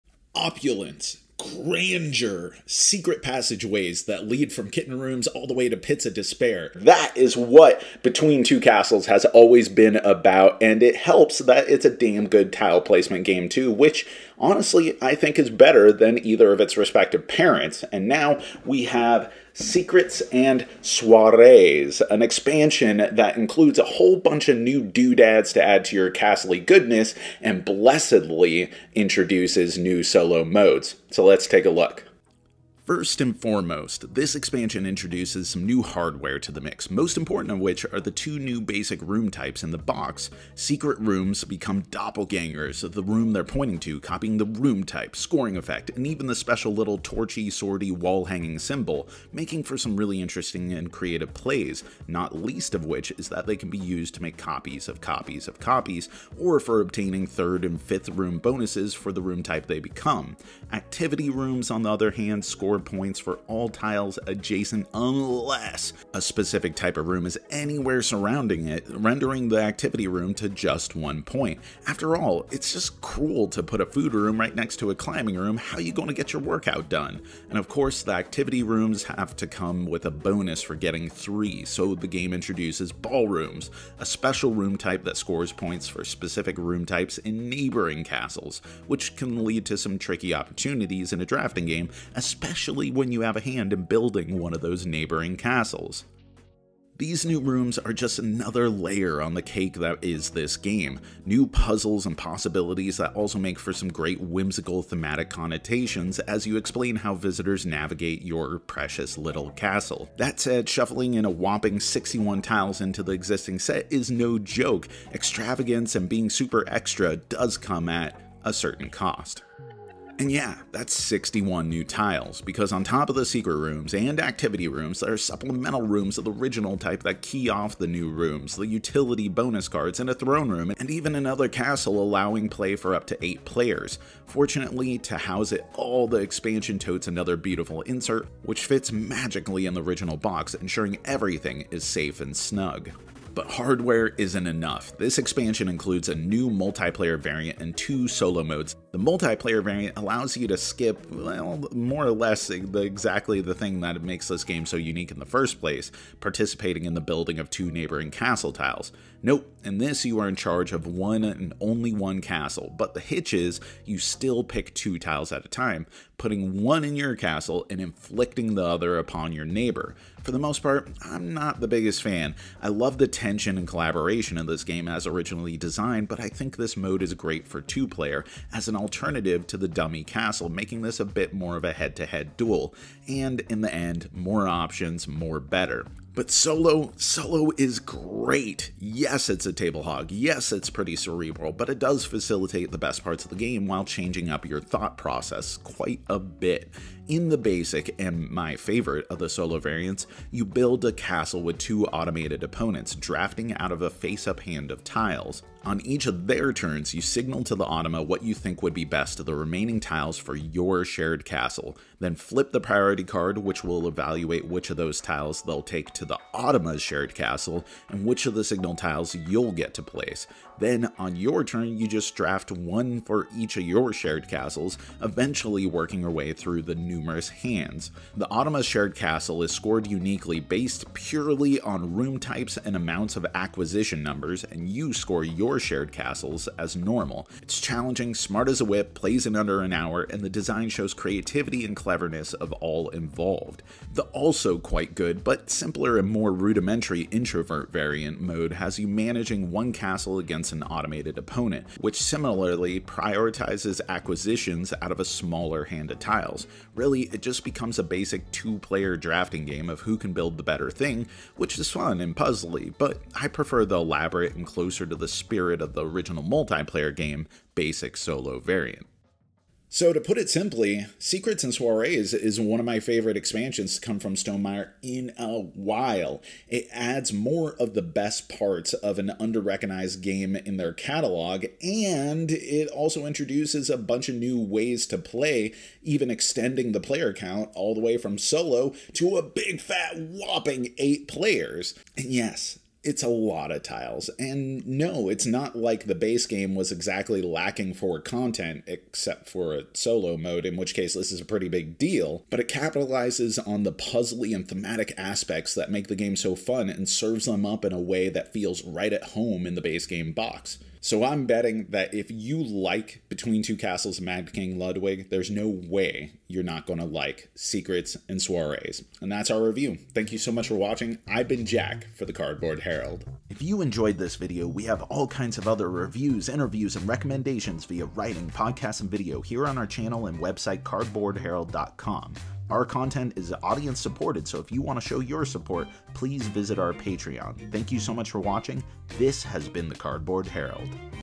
Between Two Castles of Mad King Ludwig: Secrets & Soirees Expansion Review
Secret rooms and activity rooms and solo, oh my! Come talk to us on discord